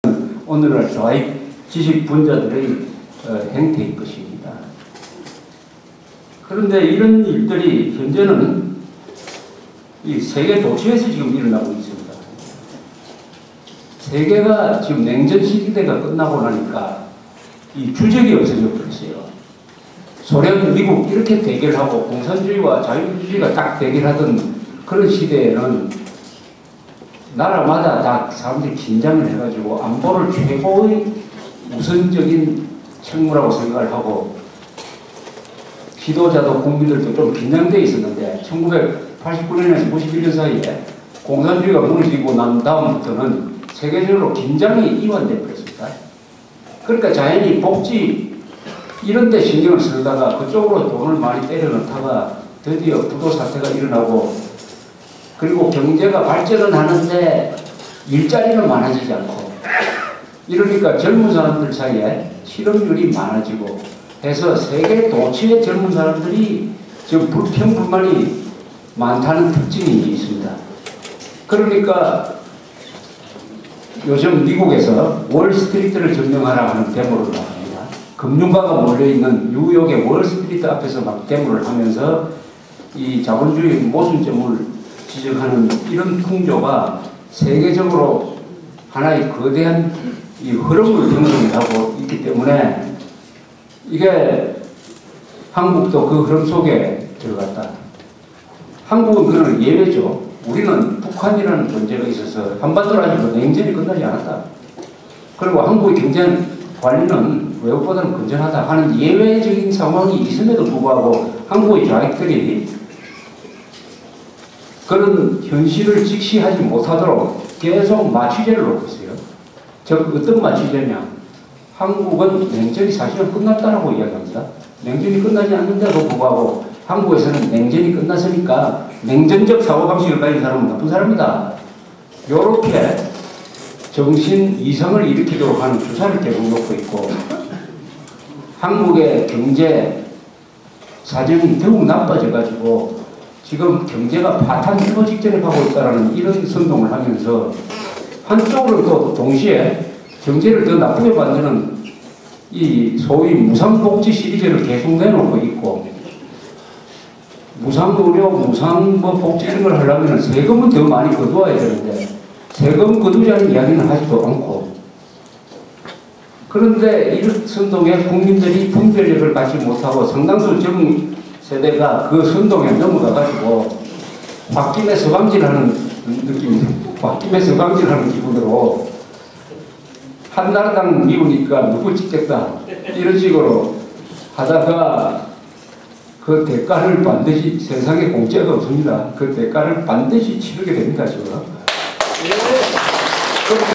[조갑제 부산 강좌 201110-11] 주적 없는 세계, 젊은이 불평불만 증대
2011년 10월29일 오후 부산일보 대강당. 조갑제 기자의 부산 월례강좌.